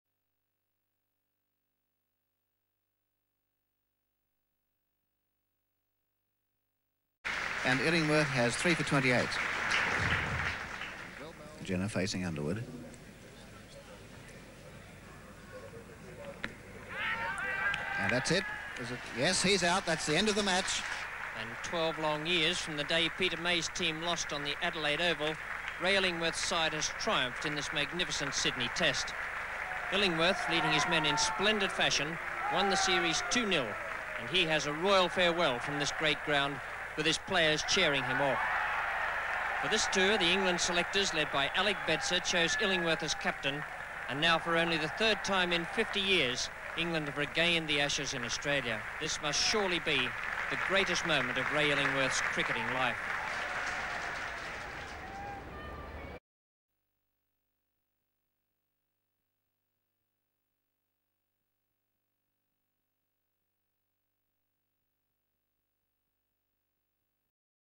TERRY JENNER c KEITH FLETCHER b DEREK UNDERWOOD 4 AUSTRALIA v ENGLAND T7 D5 SYDNEY FEBRUARY 17 1971
Needing 223 to win, Australia had reached 160-9.
This clip includes the full original commentary from Richie Benaud of Illy's "carry off".